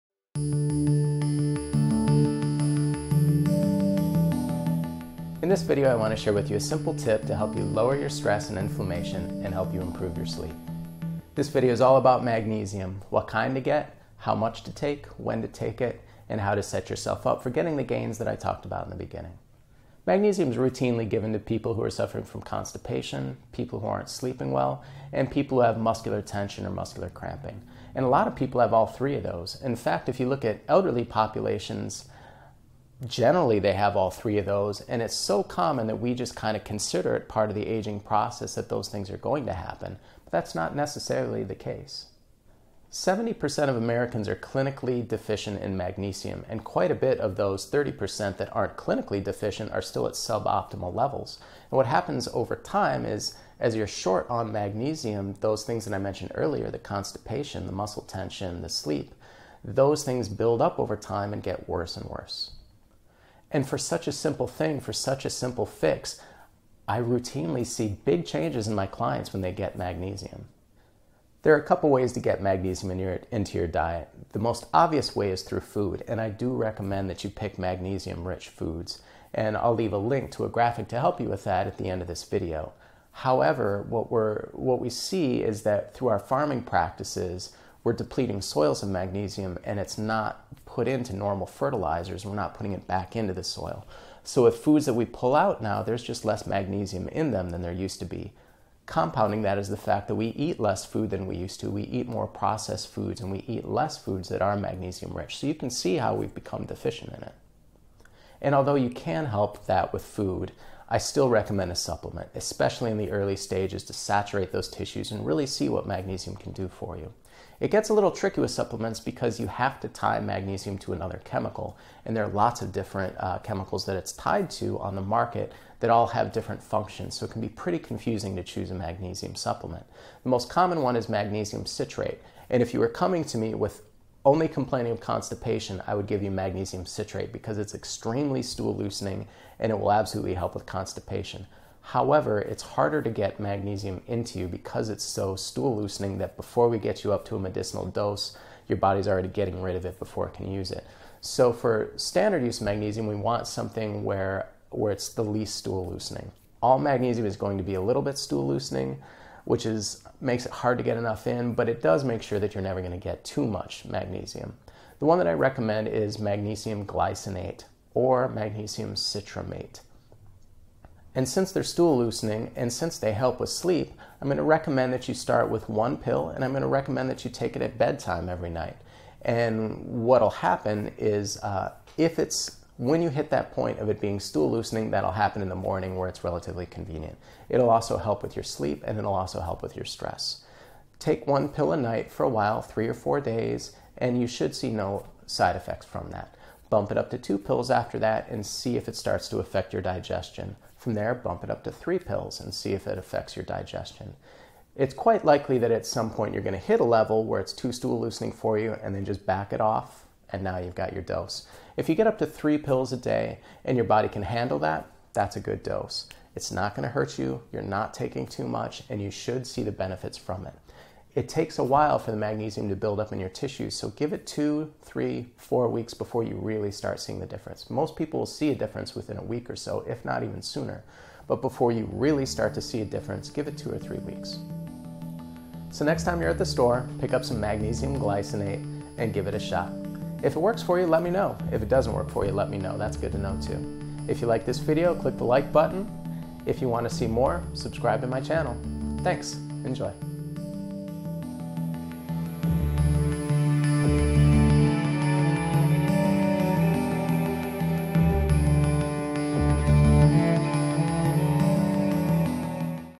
Magnesium- What You Need to Know About This Critical Nutrient
Instructions on how to take magnesium and why you might want to.